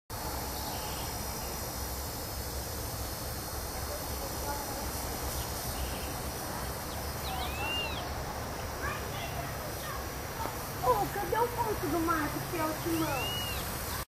Banho Passarinho
Pardais tomando banho no bebedouro do Cateto